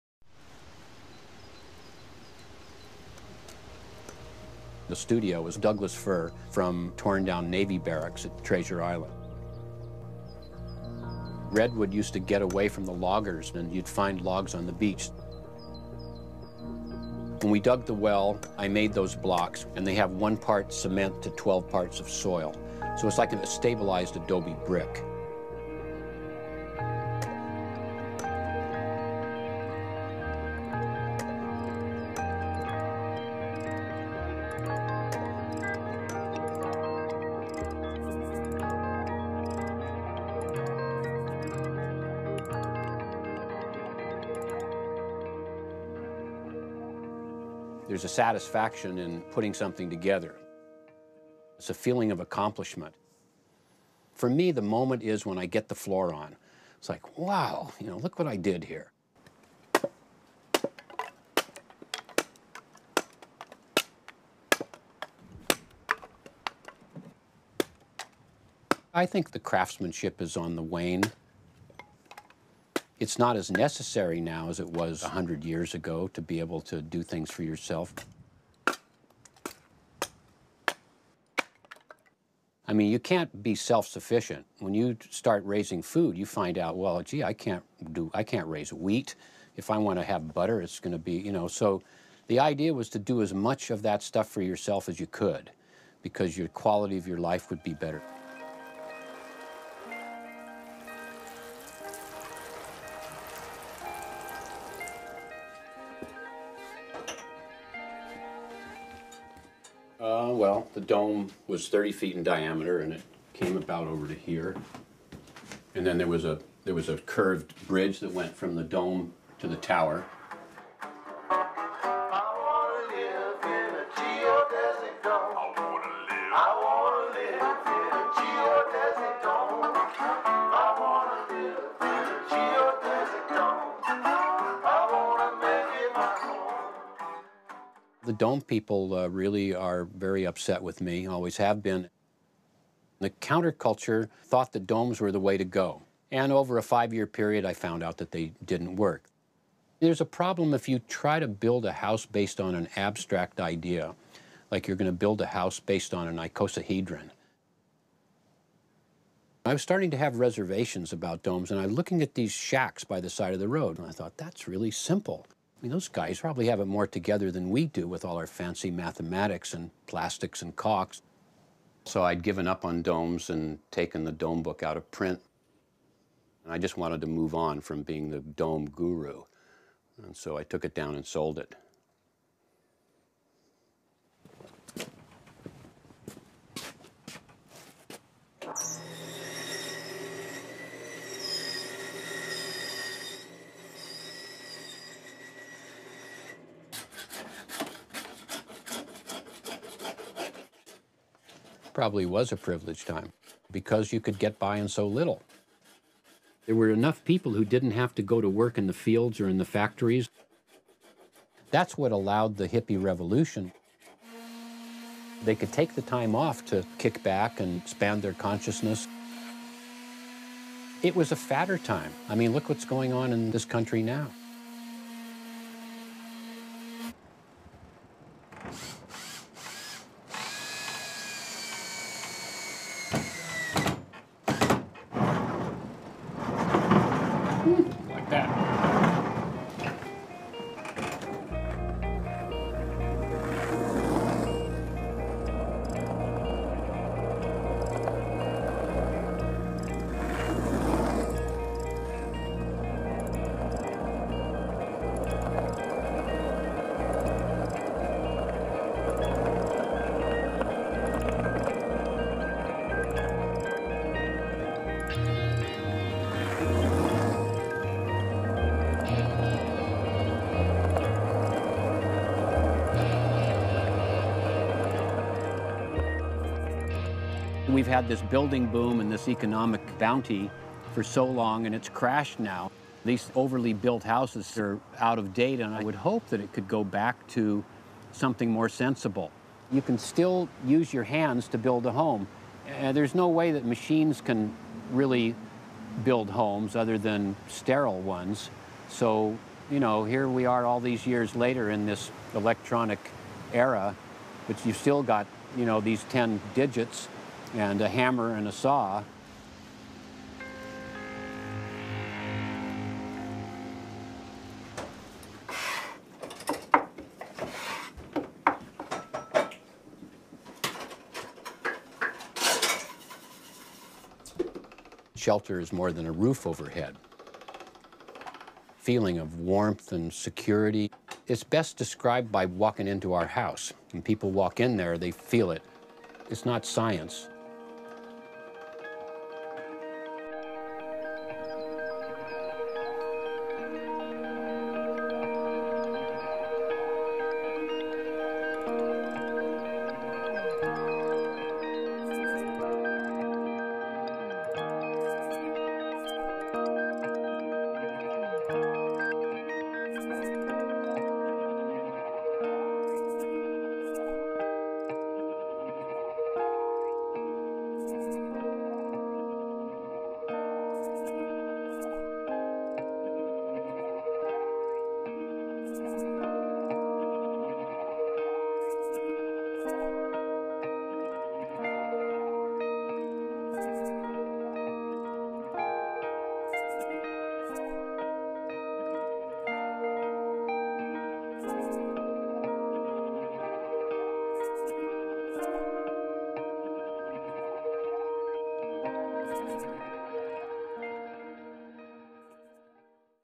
Tiny Homes: Simple Shelter (book trailer) / Lloyd Kahn
A book trailer for "Tiny Homes: Simple Shelter / Scaling Back in the 21st Century" by author Lloyd Kahn. Video produced by Jason Sussberg.